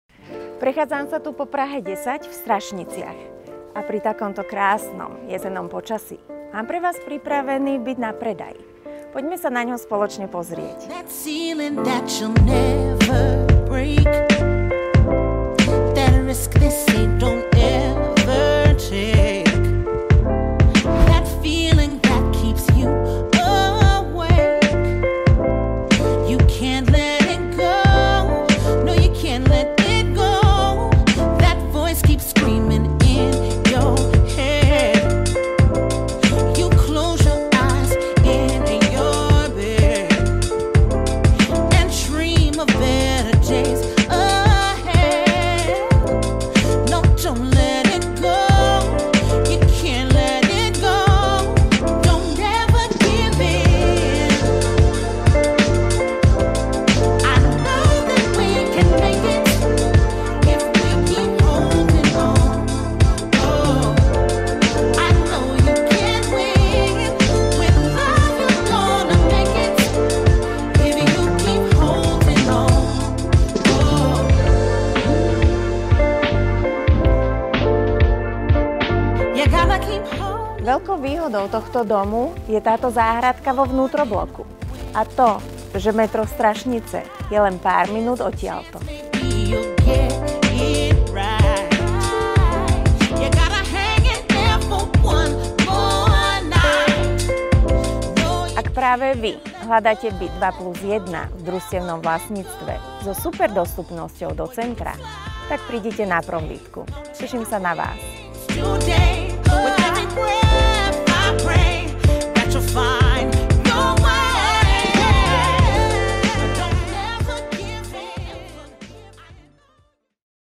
Videoprohlídka - Prodej bytu 2+1, Praha 10 - Strašnice
Nabízíme k prodeji pěkný byt 2+1 o velikosti 50m² s nebytovým prostorem o velikosti 11m2, v Praze 10 ve Strašnicích, ulice Kounická. Družstevní byt se nachází ve vyvýšeném 1. nadzemním podlaží 4 podlažního cihlového domu se zahrádkou ve vnitrobloku. Dispozici tvoří předsíň, na kterou navazuje kuchyň 8m2, obývací pokoj 23m2, ložnice 12m2 se šatníkem a prostorná koupelna 4m2 vybavená rohovou vanou a umyvadlem, WC je zvlášť. Byt prošel částečnou rekonstrukcí (koupelna, kuchyň, plastová okna). K bytu náleží nebytový prostor v suterénu, který se da využít jako fitness, atelier či dílna. Byt má i malou sklepní kóji. Výhodou bydlení je společná pěkná zahrada ve vnitrobloku, vhodná na posezení a grilování. Dům má nové stoupačky, je odizolován a podkroví bylo přebudováno na byty. Anuita na byt je komplet splacena a převod do OV momentálně není plánován. PENB nebyl dodán. Jedná se o malé družstvo jen obyvatelů domu. Podle stanov družstva se může byt dále podnajímat. V okolí domu je v pěší vzdálenosti poliklinika, škola, školka, potraviny a restaurace. Metro Strašnická, Skalka a Želivského je jen par zastávek tramvají od domu. Dům se nachází v klidné ulici. Je vhodný pro bydlení i investici pro jeho dobrou dostupnost do centra. Byt je k dispozici ihned k nastěhování. Těšíme se na Vás na prohlídce.